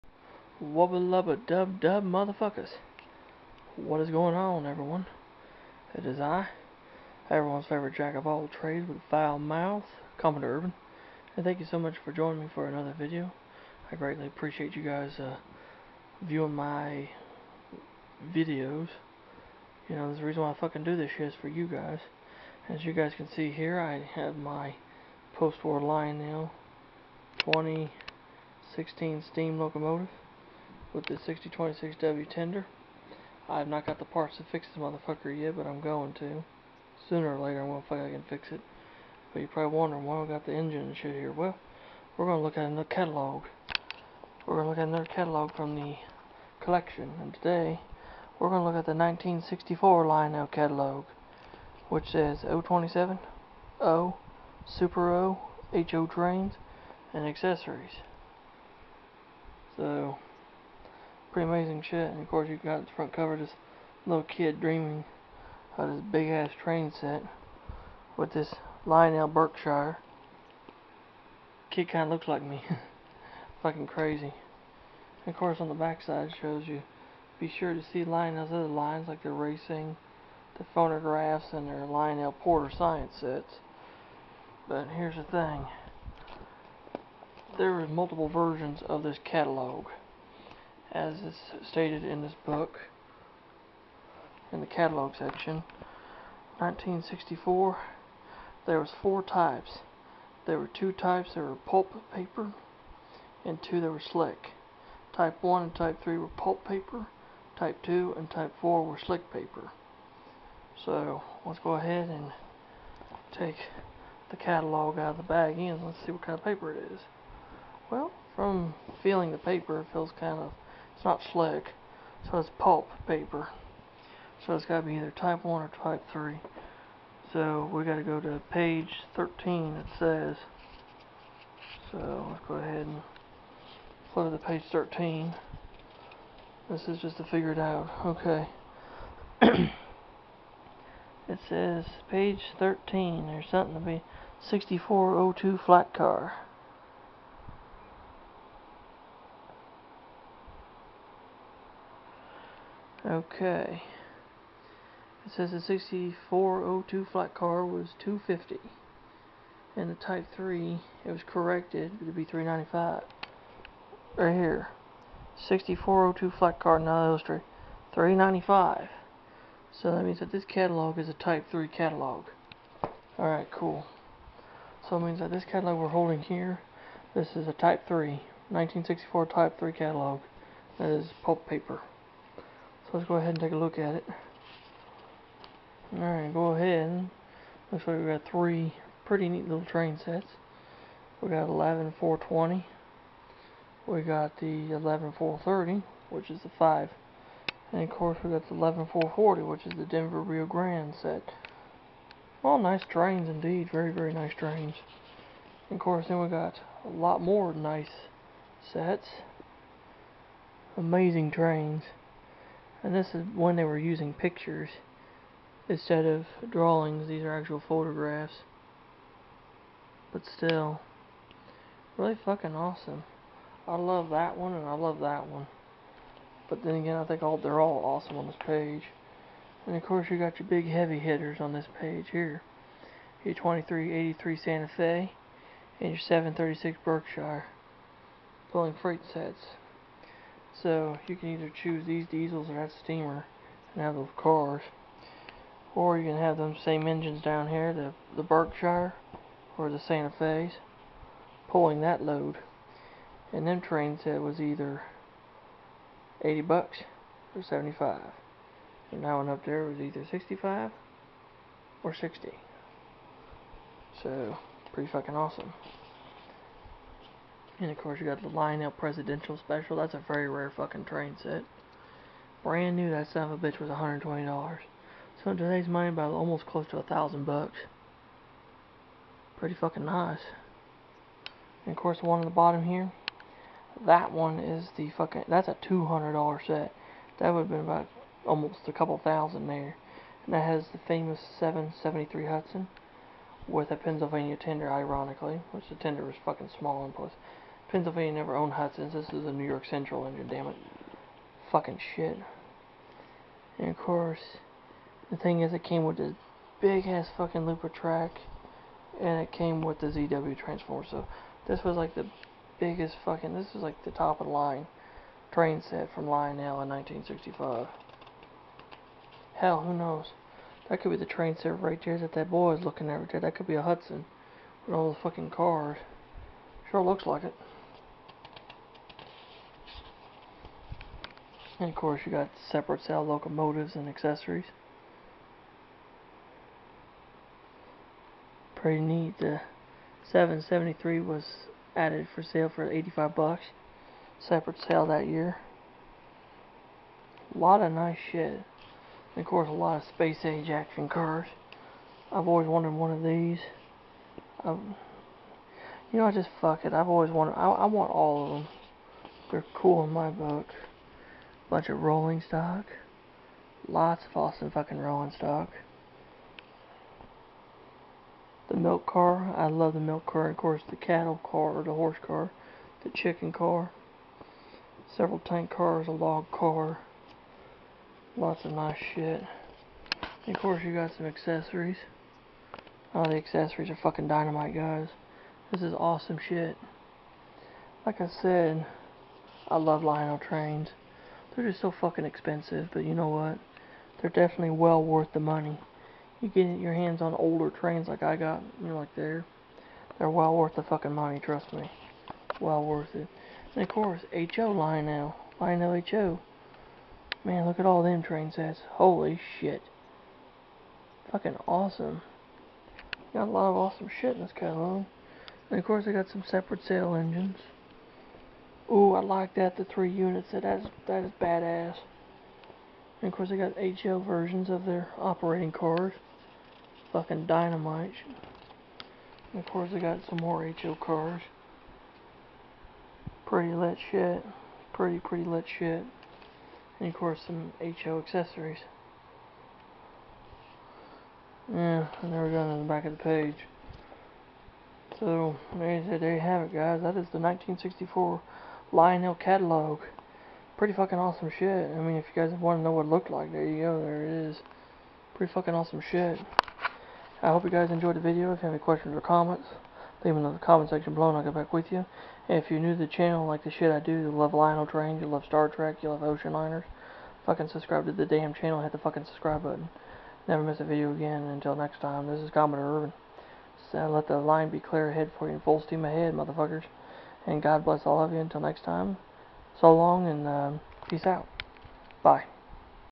Inside the 1964 Lionel catalog
I show the inside of the 1964 Lionel trains catalog from the postwar era.